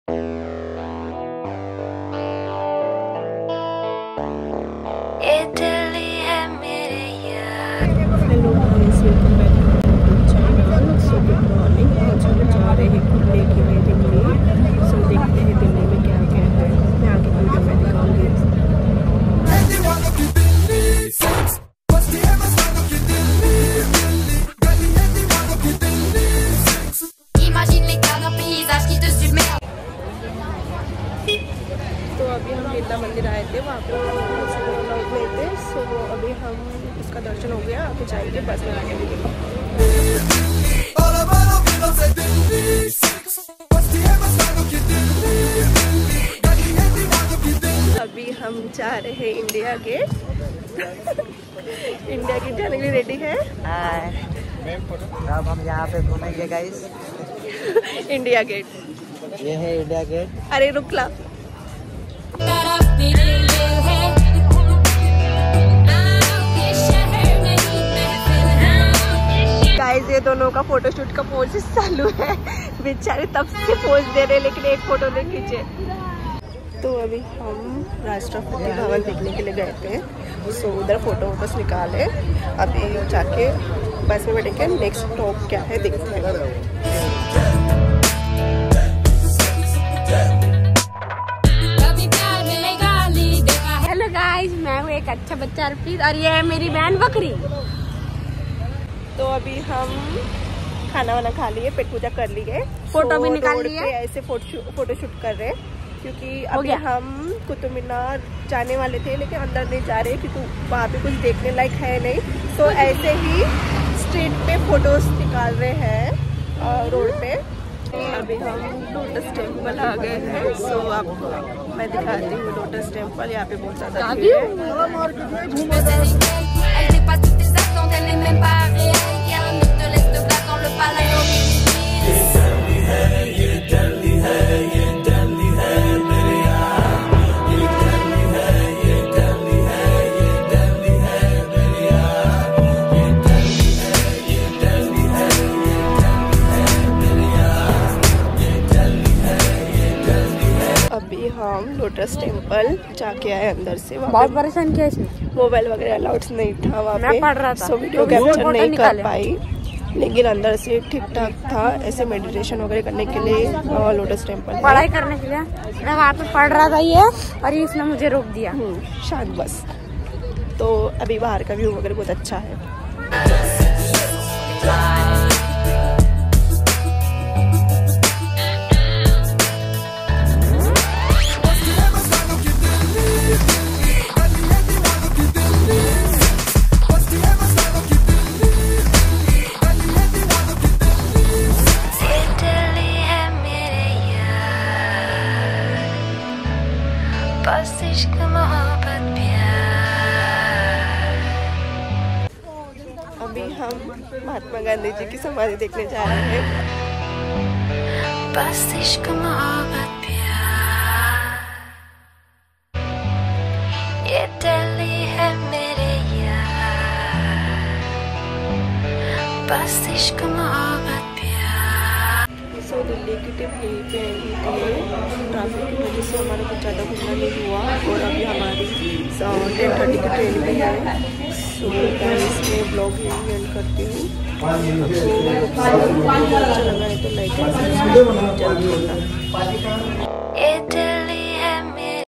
Ithne Saalo Bad Delhi Ghumne Gai🥹 | Delhi Vlog | With Family | @Thenursesvlogs | apekshawankhede
Delhi Vlog 😍
Hey guys it’s my new vlog 🥰